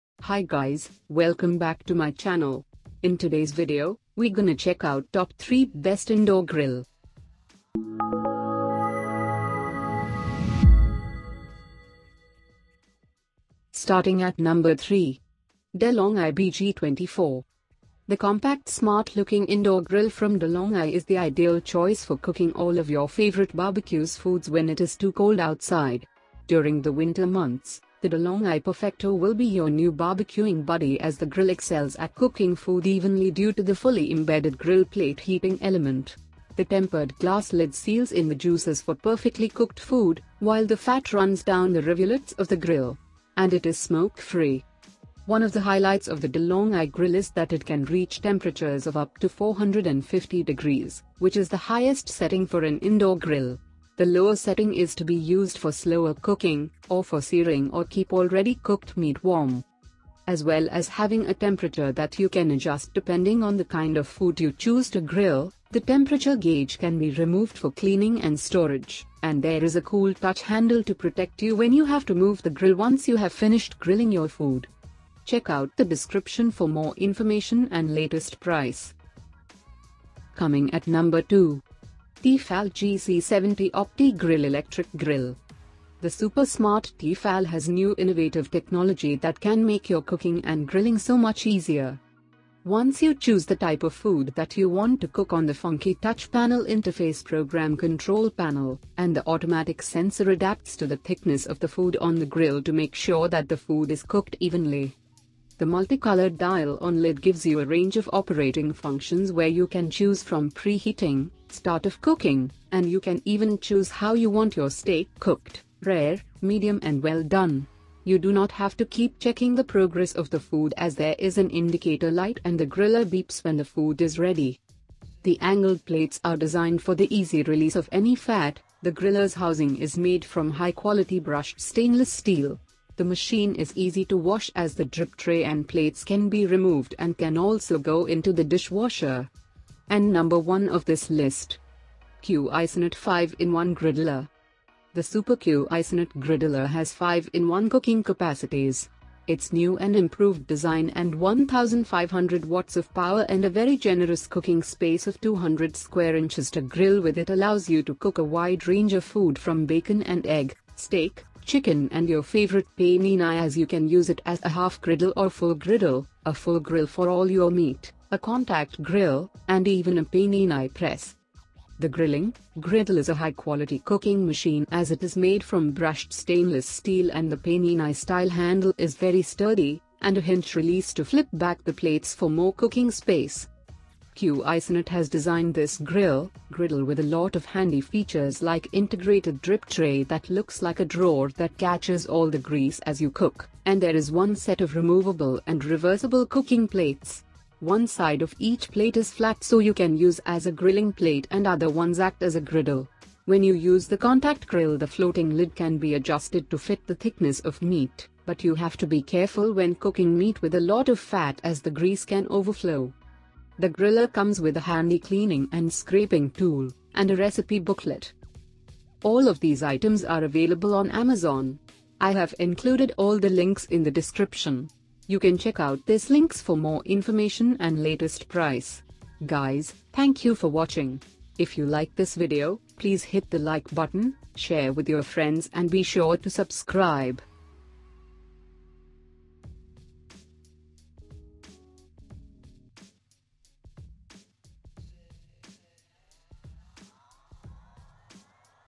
3 Best Indoor Grill in 2023
Here is the Updated Prices of the Best Indoor Grill in 2023!

Things that we mentioned in this video:
1. Cuisinart 5 in 1 Griddler - B002YD99Y4
2. T fal GC70 OptiGrill Electric Grill - B00H4O1L9Y
3. Delonghi BG24 - B0000AZK85